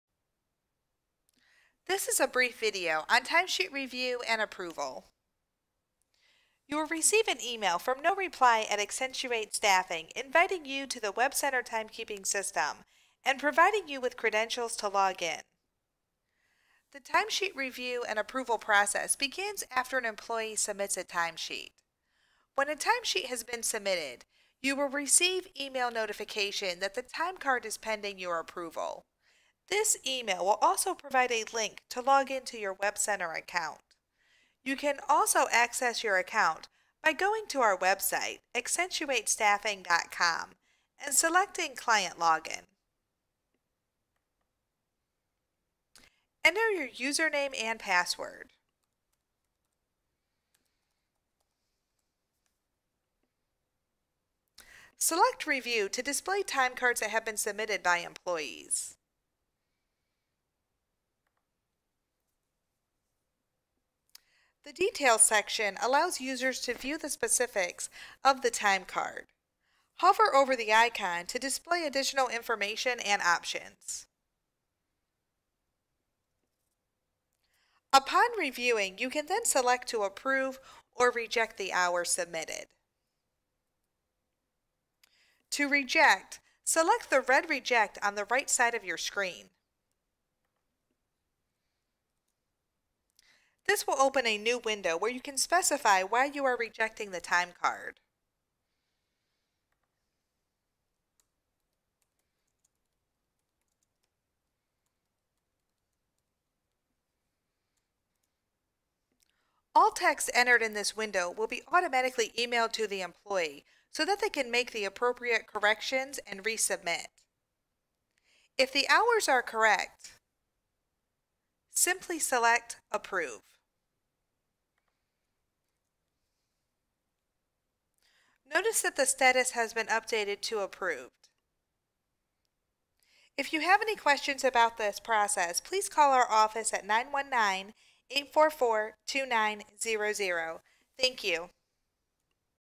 Supervisor - Approving Timecards in Webcenter
This video shows the steps that supervisors will take to approve time that has been submitted in WebCenter.